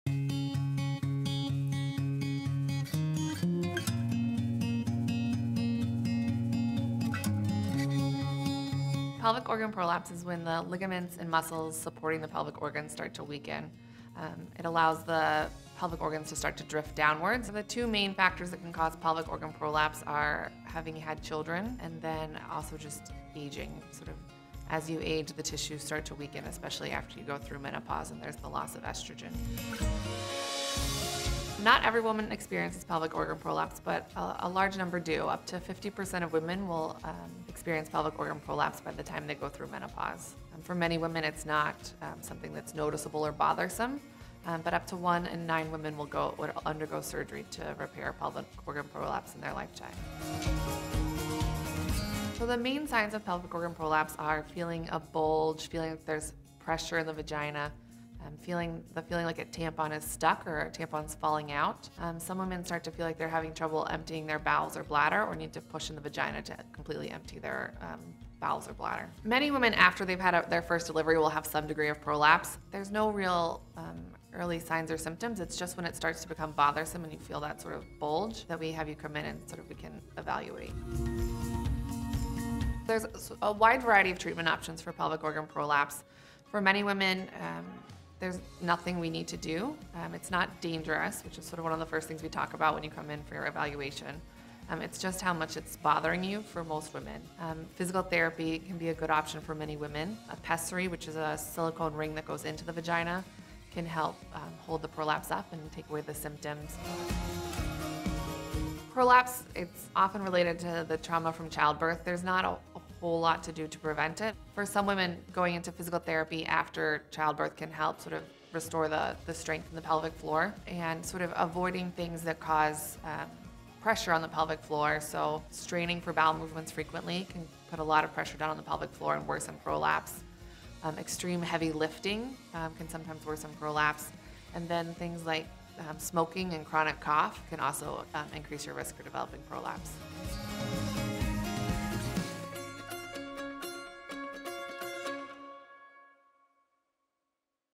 Katherine Armstrong - Fast5 - Pelvic Organ Prolapse
Nearly 50 percent of women will experience pelvic organ prolapse by the time they go through menopause. Katherine Armstrong, MD explains the signs, symptoms and treatment options available.